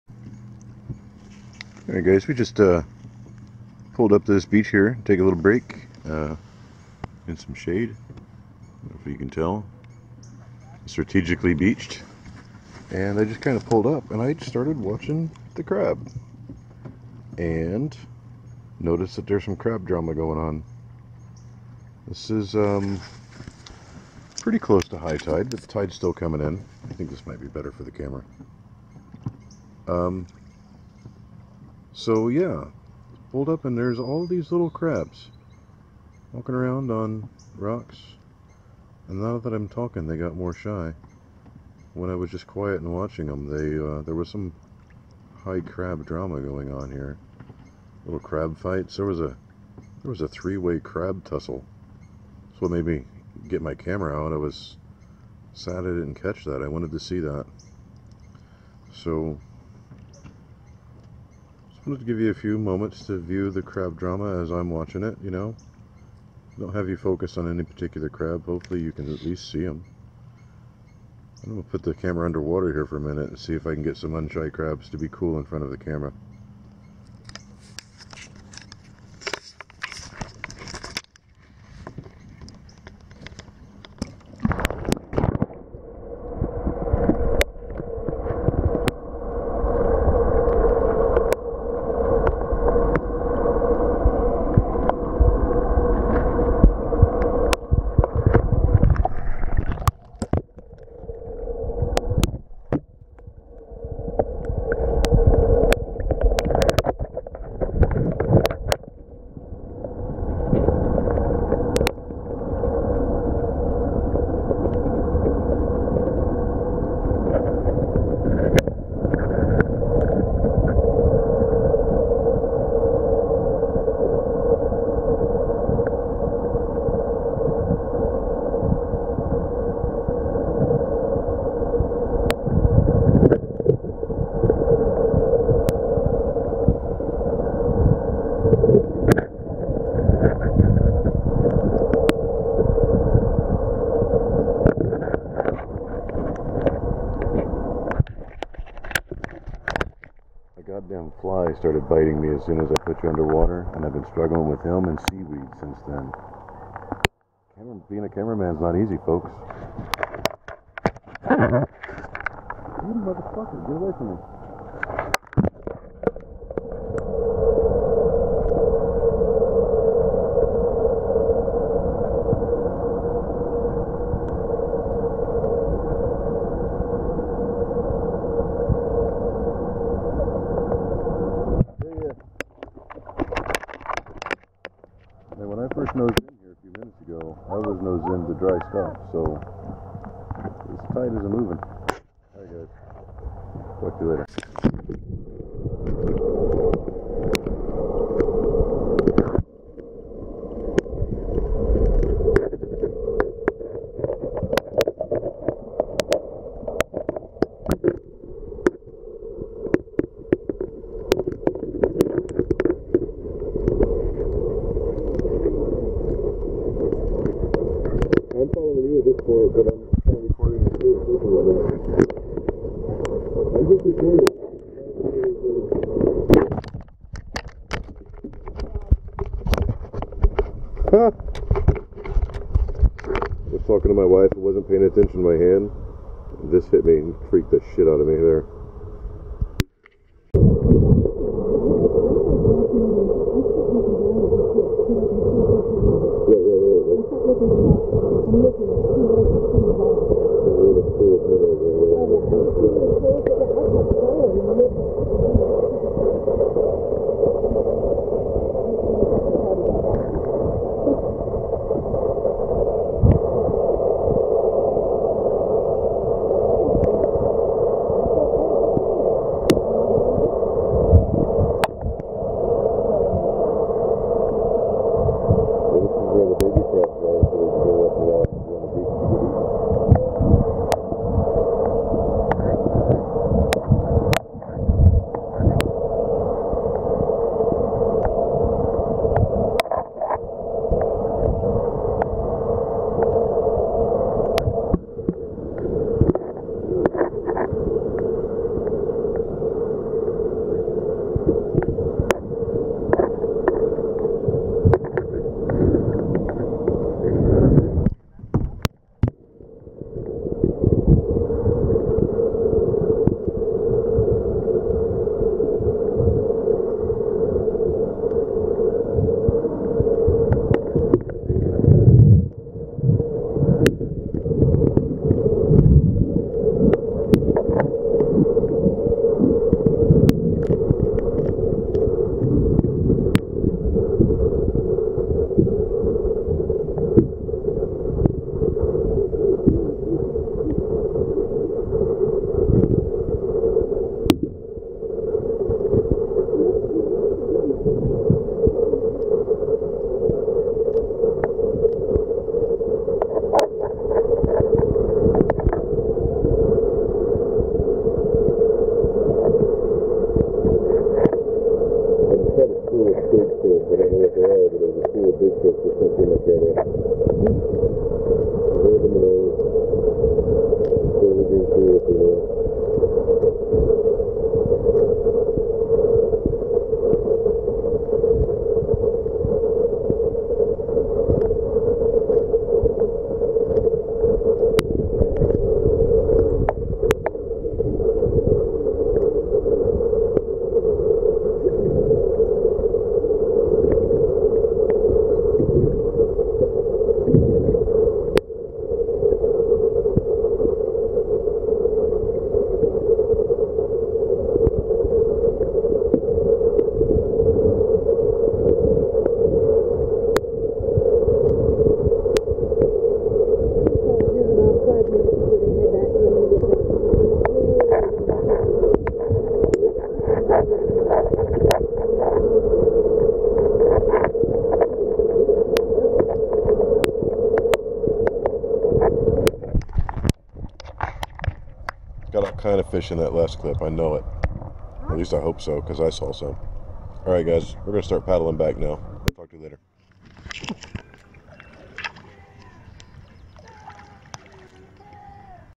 Elger Bay Crabs
Checking out Elger bay near high tide.  Some cool underwater footage.  Crab drama.  And a biting fly.